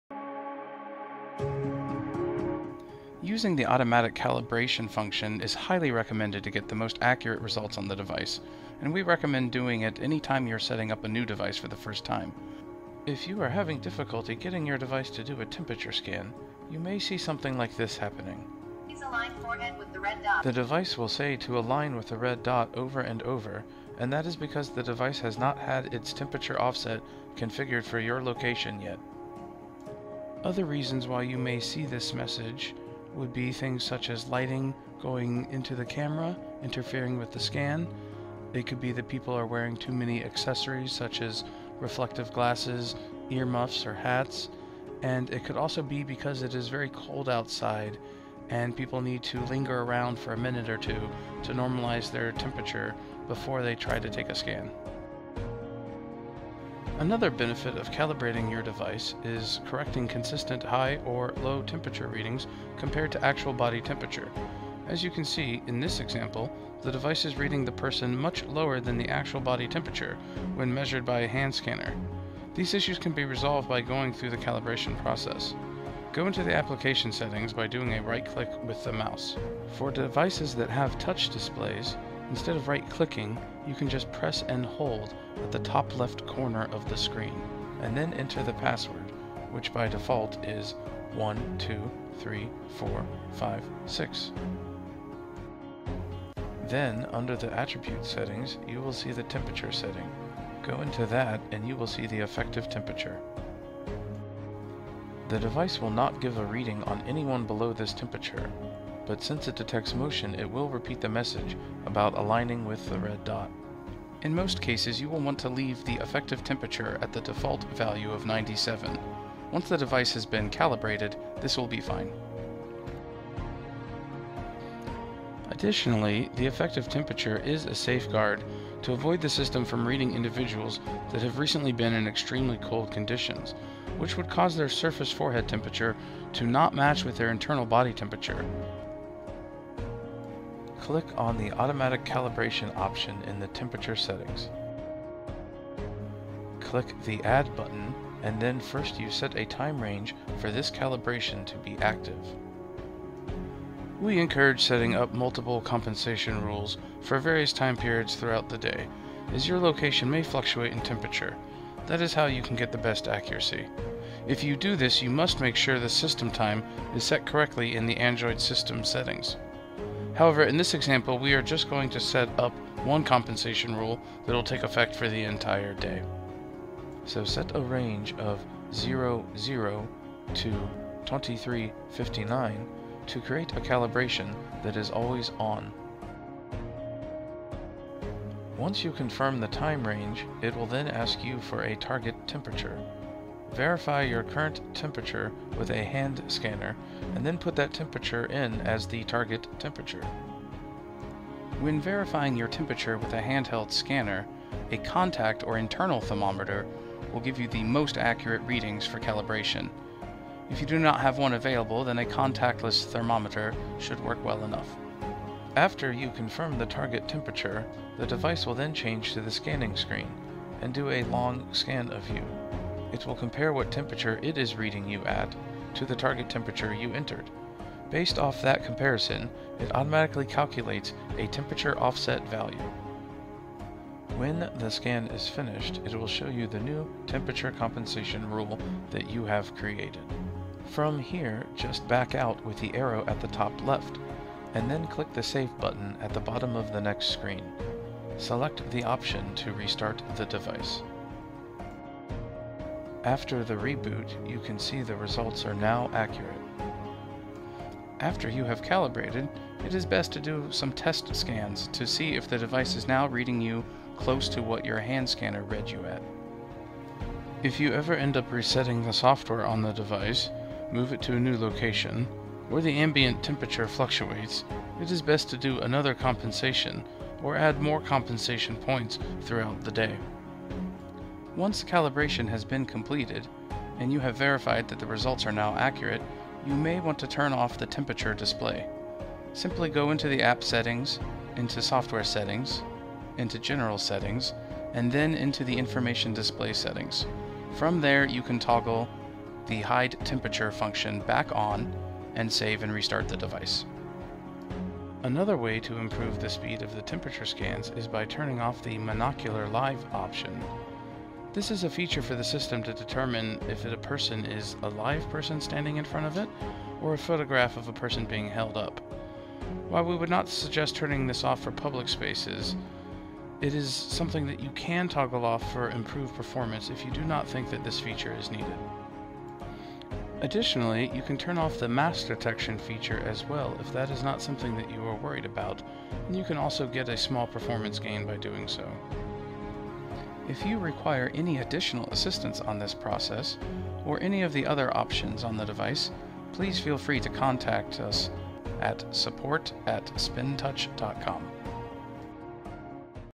Calibrating your device | RapidScreen
Investing in a thermal scanning kiosk such as RapidScreen by spinTouch will help maintain a safe environment by reducing risk and limiting access to those indicated with a fever.

In this video you will learn how to calibrate your RapidScreen device in just minutes.

If you have further questions,